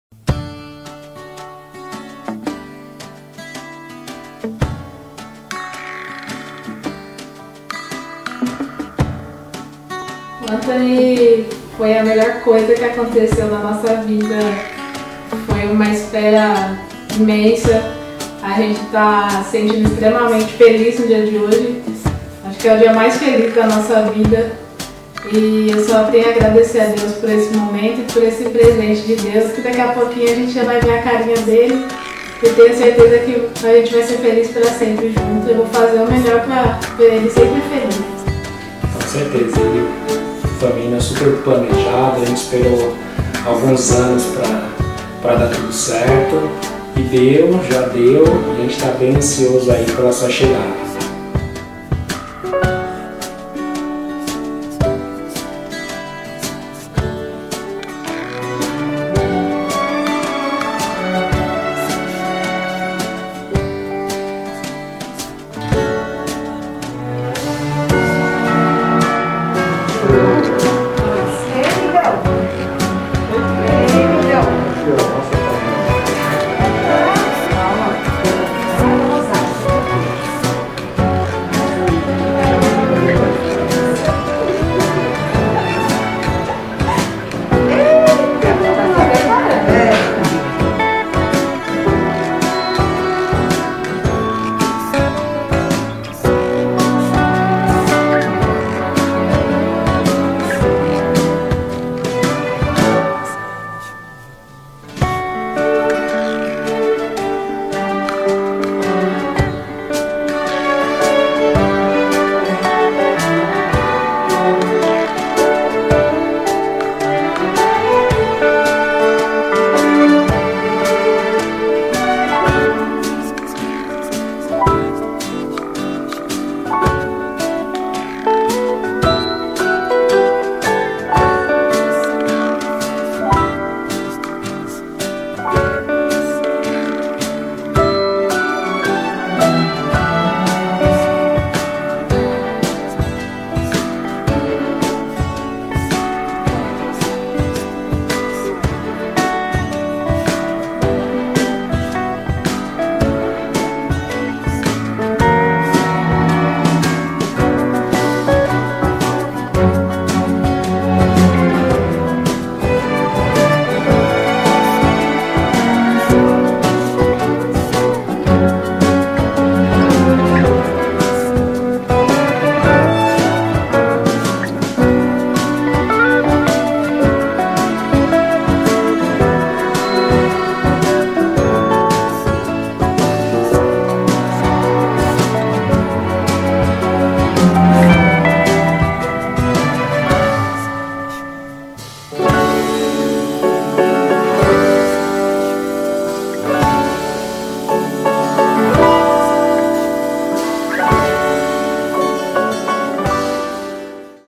Nascimento do Anthony
Dia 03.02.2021, foi a data de darmos boa vindas ao Anthony 😍